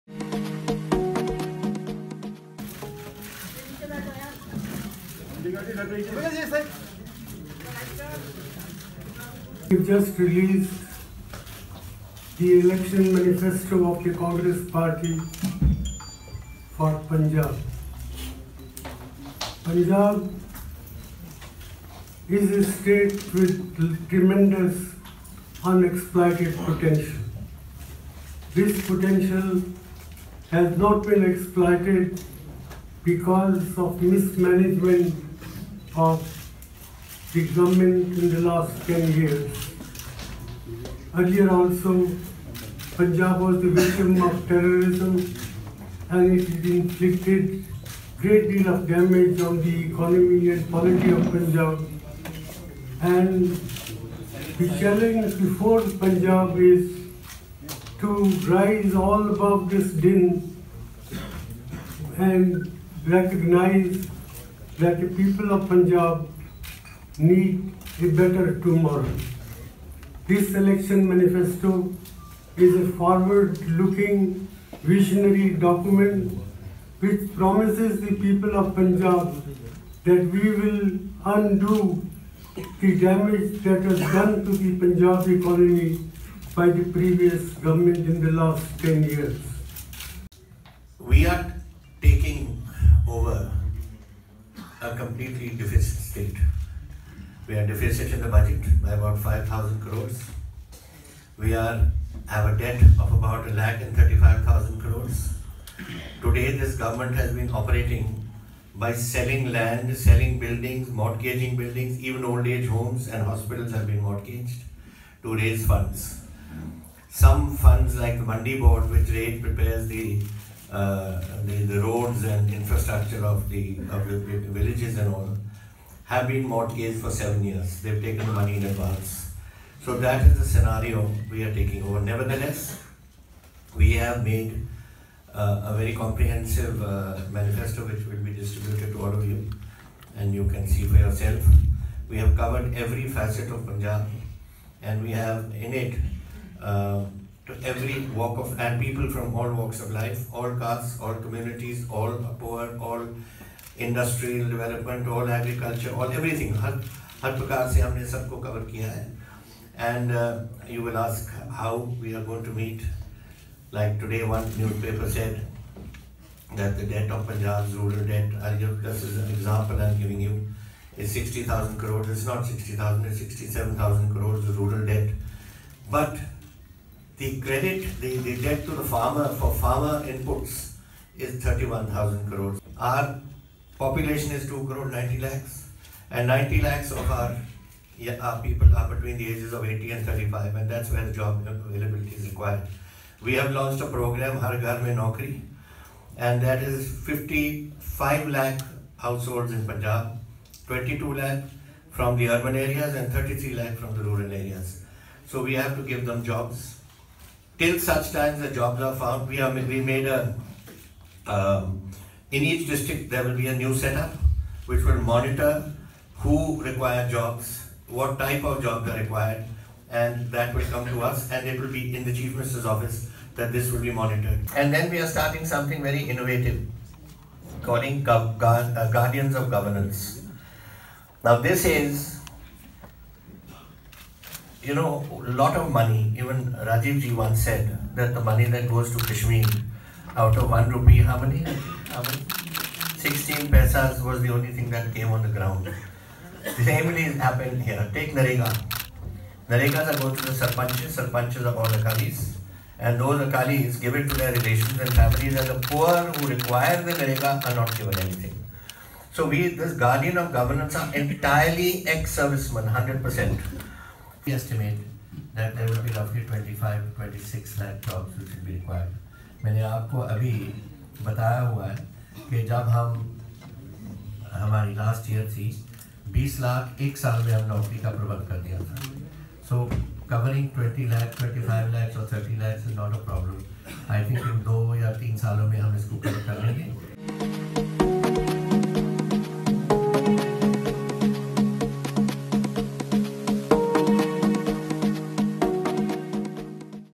Manmohan Singh releases Congress's Punjab manifesto 2017
Video by Tribhuvan Tiwari/Outlook